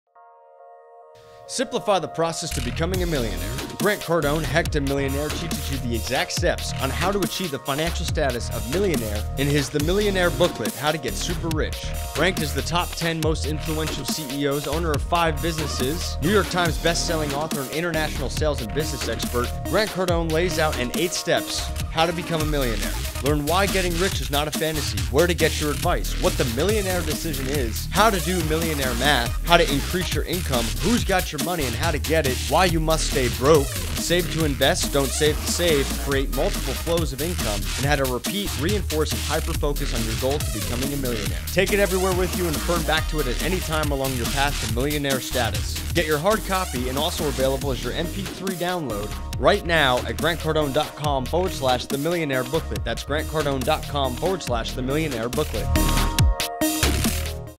How to Get Super Rich - Grant Cardone
Have you ever wondered how to get super rich?  Well New York Times Best Selling Author and International Sales and Business Expert Grant Cardone will show you how in 8 steps.  Grant Cardone's The Millionaire Booklet How to Get Super Rich is available right now in hard cover and mp3 download.

Get The Millionaire Booklet and The Millionaire Booklet MP3 together for one special price!

The eight steps Grant Cardone lays out is in a very simple to understand language that will allow you to get started today in creating the money you deserve. Let’s face it, your parents didn’t teach you how to get rich and the schools and colleges don’t even talk about it.

Grant teaches you:
Getting Rich Is Not a Fantasy
Where You Get Your Advice
The Millionaire Decision Chapter
Millionaire Math
Increase Income
Who’s Got My Money?
Stay Broke
Save to Invest, Don’t Save to Save
Multiple Flows of Income
Repeat, Reinforce and Hyperfocus